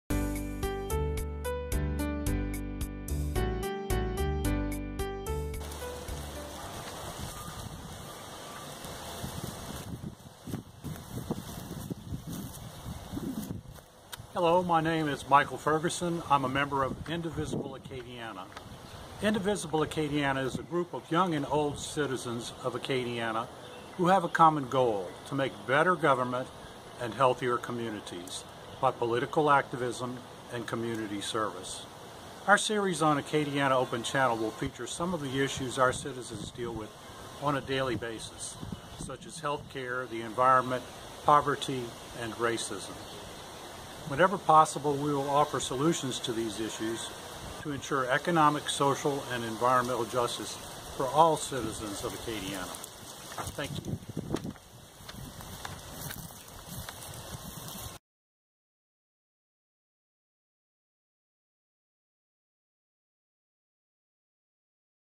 What is Indivisible Acadiana?
Indivisible Acadiana is a local group of citizens who are working to insure good quality government representation and economic, social and environmental justice for all citizens of Acadiana.